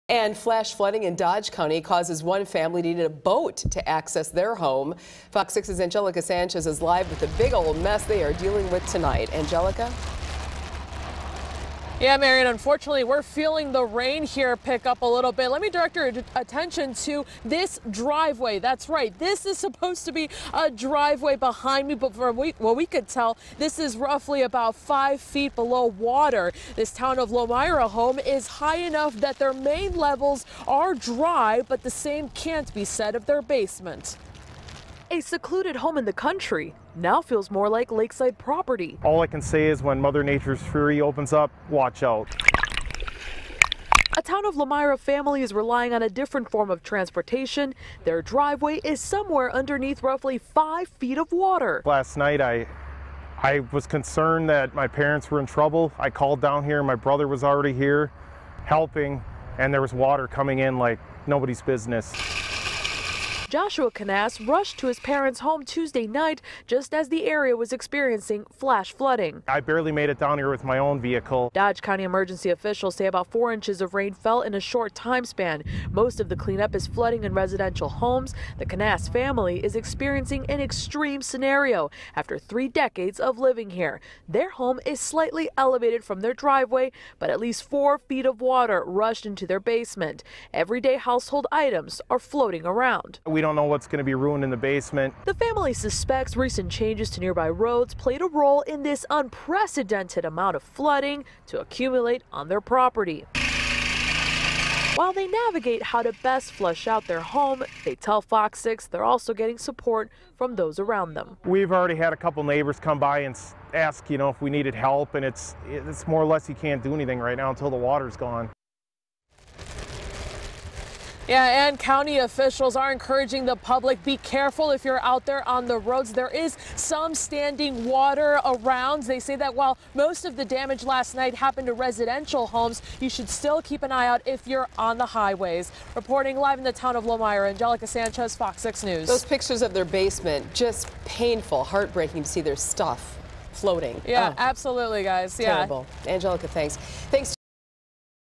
Heavy rain swamps Town of Lomira home
A Dodge County home was hit so hard by Tuesday night's storm that the family's driveway was under at least five feet of water Wednesday, Oct. 2. In fact, our crews needed a small boat to even access the front entrance at the home.